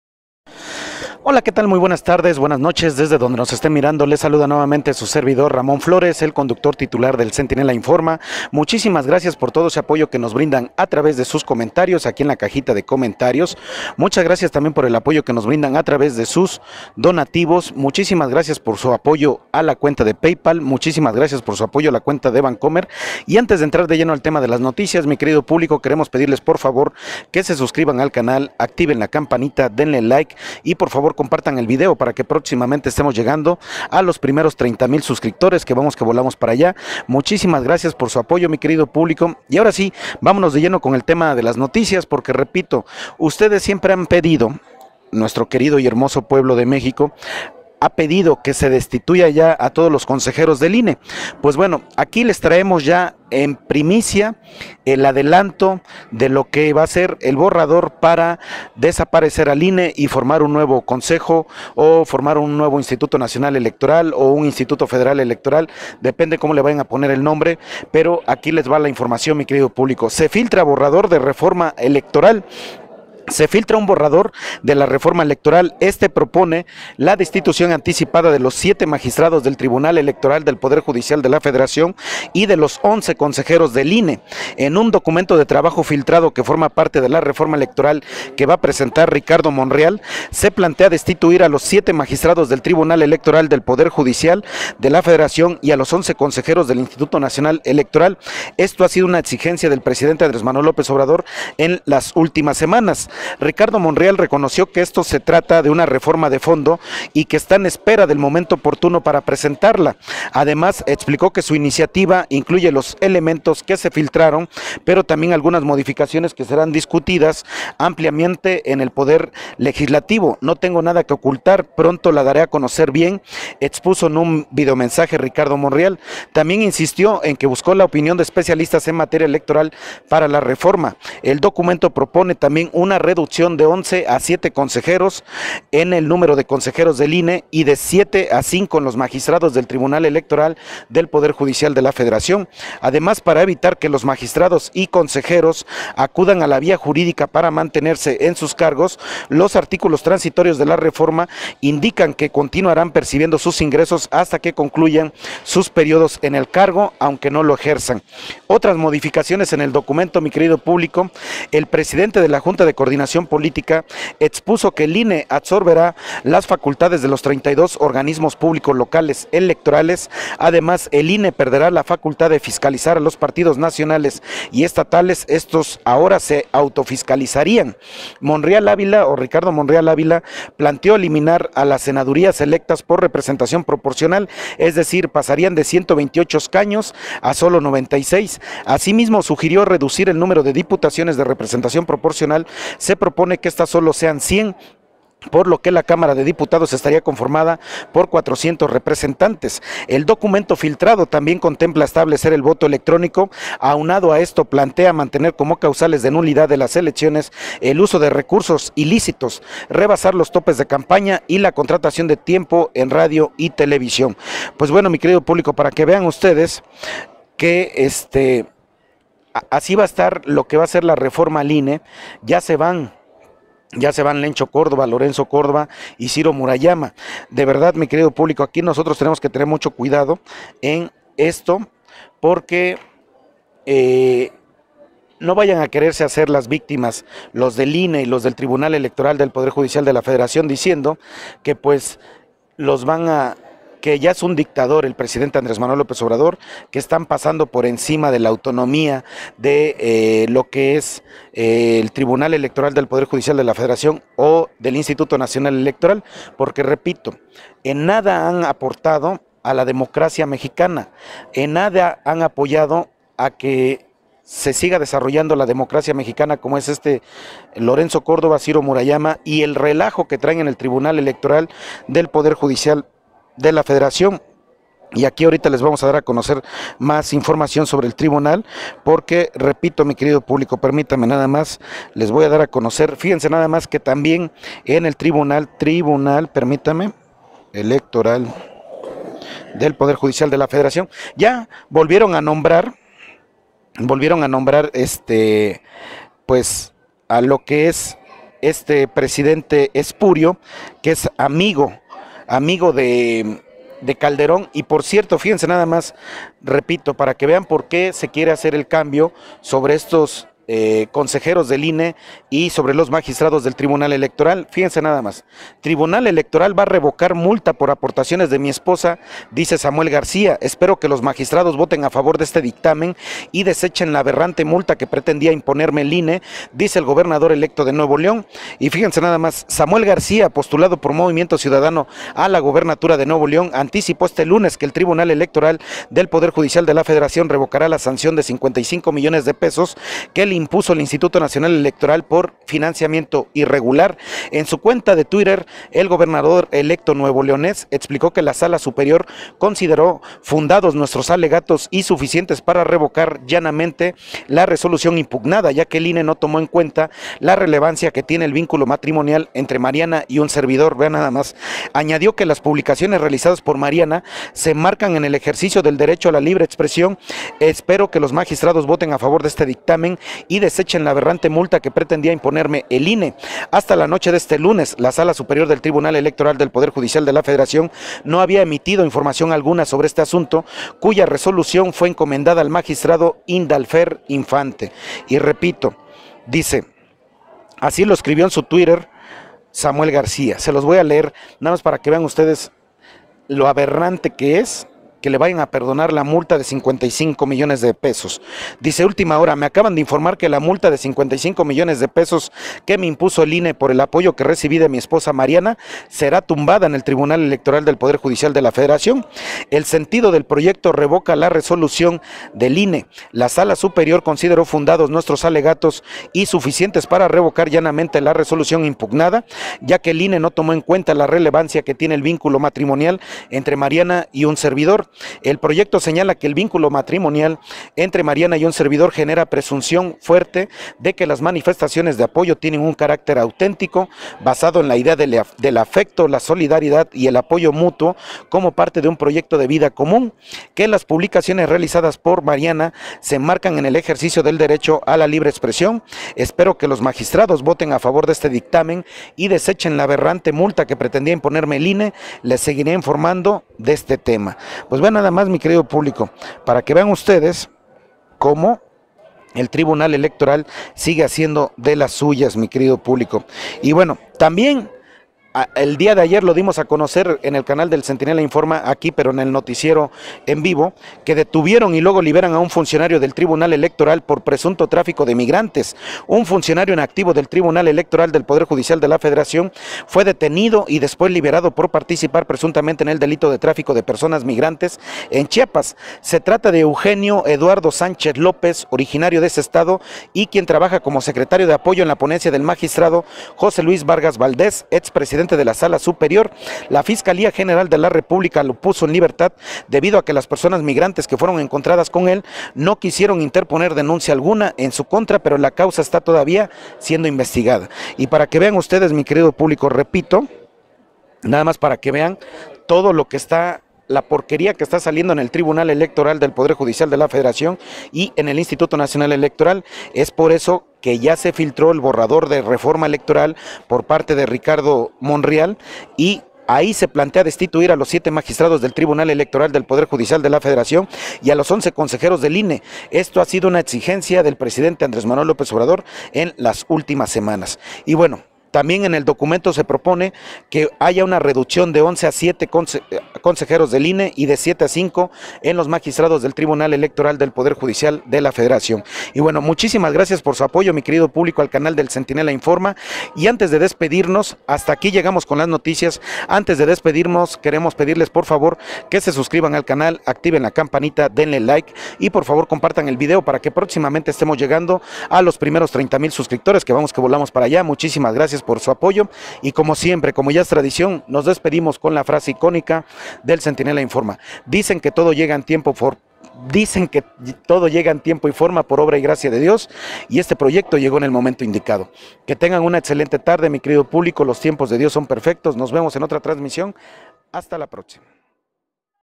¡Filtran Información y el TEPJF y el INE ya valieron Madr3, entérate aquí!
¡Aquí les traemos todos los detalles de como el TEPJF Y EL INE, van a desaparecer, entérate de todos los detalles aquí!